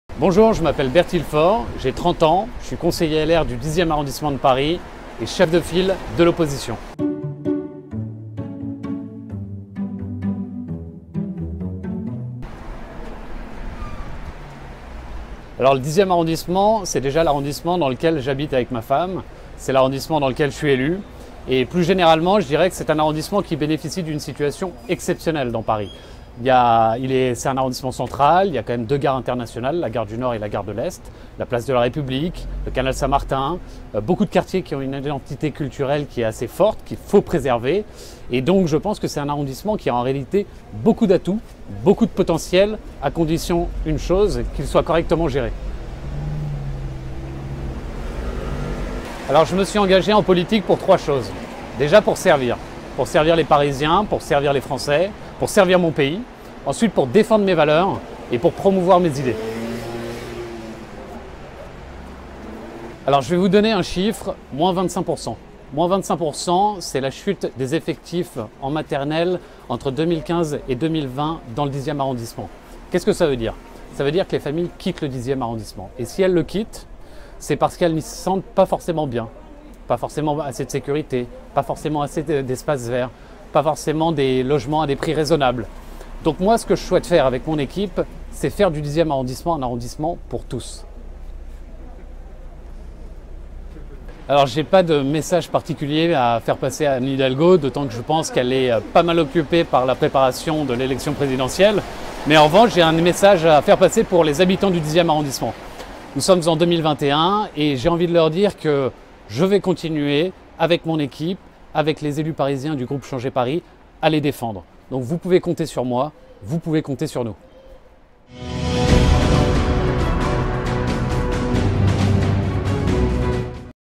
Les nouveaux visages de la droite parisienne : Bertil FORT
Notre fédération s’est inscrite dans une dynamique positive d’émergence des nouveaux talents. Grâce à l’impulsion donnée par Rachida DATI, les élus parisiens de notre famille politique ont été largement renouvelés lors des dernières élections municipales.

Chaque mois, nous ferons connaissance avec l’un des nouveaux visages de la droite parisienne. Jeunes élus et/ou premiers mandats, ces hommes et ces femmes d’engagements ont fait le choix de servir les parisiens pour les six prochaines années.

Nous rencontrons aujourd’hui Bertil FORT, nouvellement élu conseiller du 10ème arrondissement.